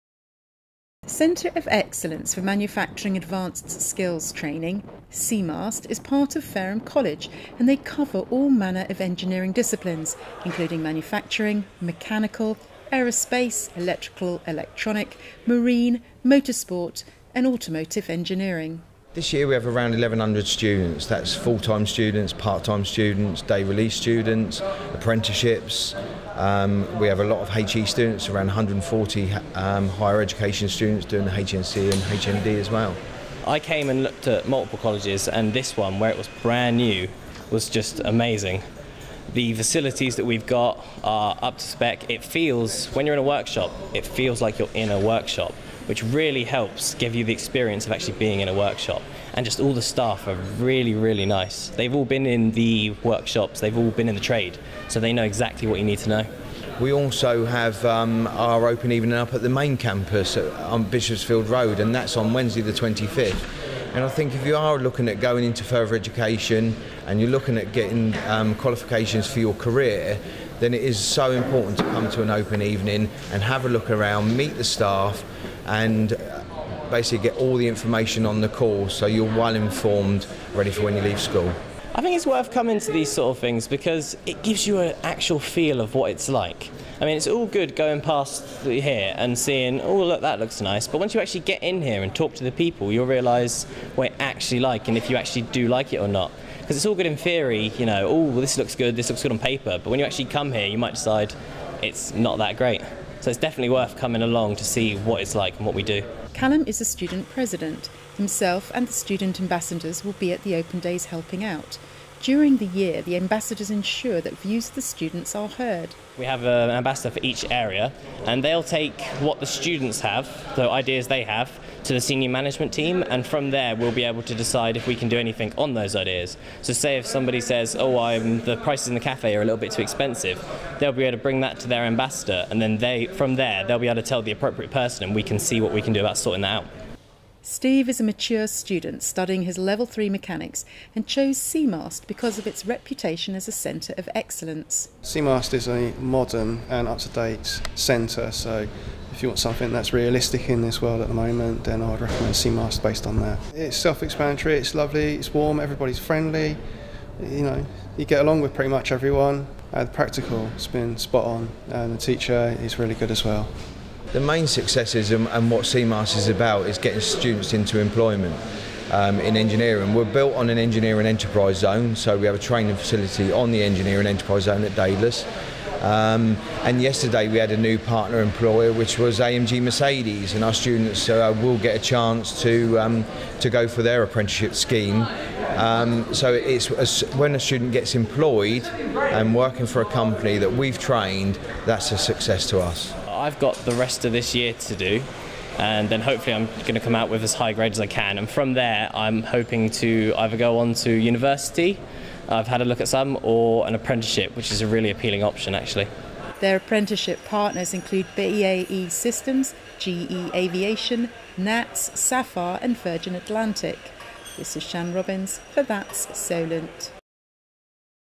Behind The Scenes At Fareham's Engineering Training Centre, CEMAST
Fareham College and it's engineering centre, CEMAST, will be holding open evenings next week. CEMAST was only opened last year and our reporter Shan Robins went behind the scenes at in Lee-on-Solent to find out more.